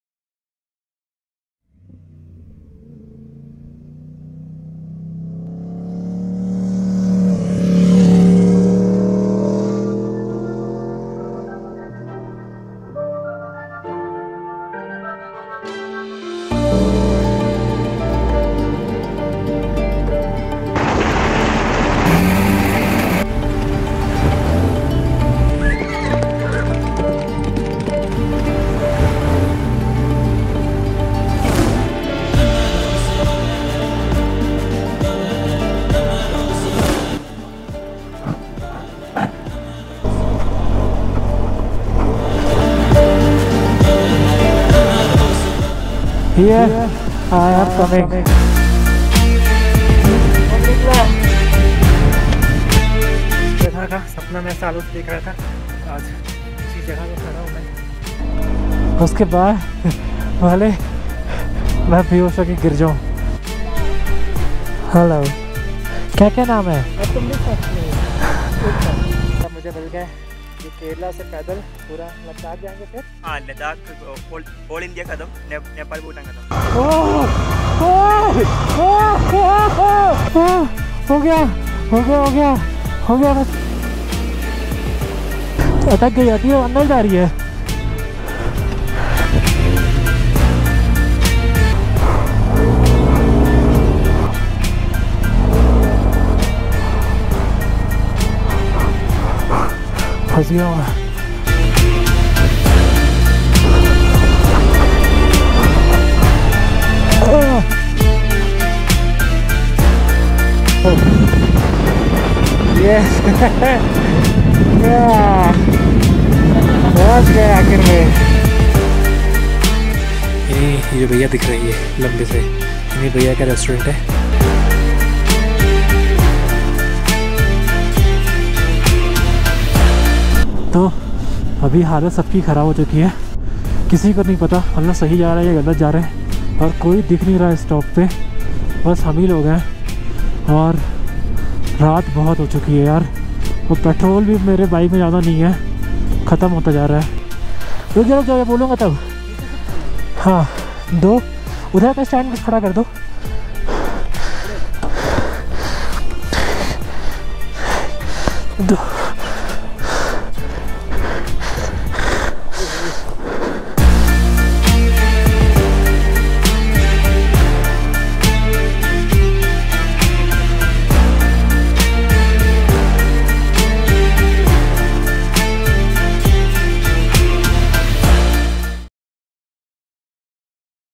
LADAKH ROAD TRIP | Varanasi to Ladakh on Benelli trk 502x | Solo Ladakh Ride | LADAKH TRAILER
Embark on an epic solo Ladakh road trip like never before! Join us on this thrilling adventure as we ride from the spiritual city of Varanasi to the breathtaking landscapes of Ladakh on the powerful Benelli TRK 502X. Get ready to witness the mesmerizing beauty of the Himalayas, conquer challenging terrains, and experience the joy of solo travel. From serene monasteries to high mountain passes, this Ladakh trailer gives you a glimpse of the unforgettable journey that awaits.